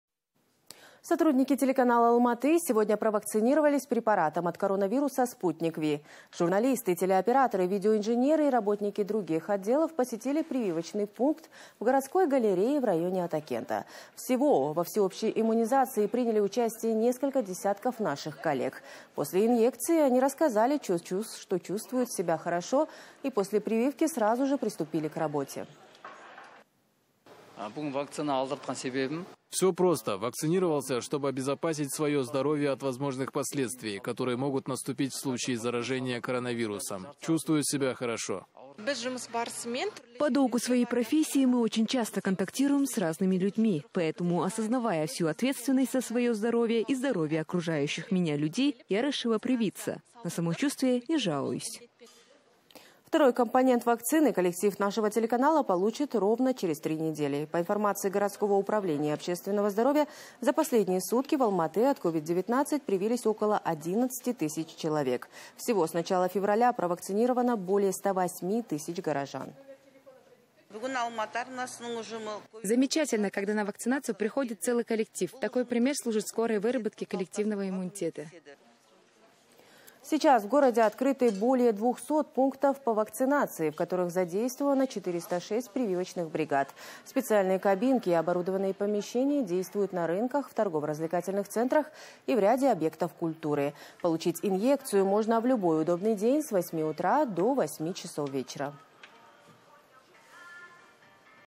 Сотрудники телеканала «Алматы» привились от COVID-19 (15.04.21)
Сотрудники телеканала «Алматы» провакцинировались препаратом от коронавируса «Спутник V», передает Almaty.tv.

Журналисты, телеоператоры, видеоинженеры и работники других отделов посетили прививочный пункт в городской галерее в районе «Атакента».

Всего во всеобщей иммунизации приняли участие несколько десятков наших коллег. После инъекции они рассказали, что чувствуют себя хорошо и сразу же приступили к работе.

«Все просто! Вакцинировался, чтобы обезопасить свое здоровье от возможных последствий, которые могут наступить в случае заражения коронавирусом. Чувствую себя хорошо», - поделился телеоператор Сагынгали Мунайтбасов.

«По долгу своей профессии мы очень часто контактируем с разными людьми. Поэтому осознавая всю ответственность за свое здоровье и здоровье, окружающих меня людей я решила привиться. На самочувствие не жалуюсь», - объяснила журналист Жанар Ганикызы.

Второй компонент вакцины коллектив нашего телеканала получит через ровно три недели.

По информации городского Управления общественного здоровья, за последние сутки в Алматы от COVID-19 привились около 11 тысяч человек.

Всего с начала февраля провакцинировано более 108 тысяч горожан.

«Замечательно, когда на вакцинацию приходит целый коллектив. Такой пример служит скорой выработке коллективного иммунитета», - отметила старшая медсестра городской поликлиники № 17 Бахарам Бектурова.

Сейчас в городе открыты более 200 пунктов по вакцинации, в которых задействовано 406 прививочных бригад. Пункты находятся на рынках, торгово-развлекательных центрах и в ряде объектов культуры.

Получить инъекцию можно в любой удобный день с 8:00 до 20:00.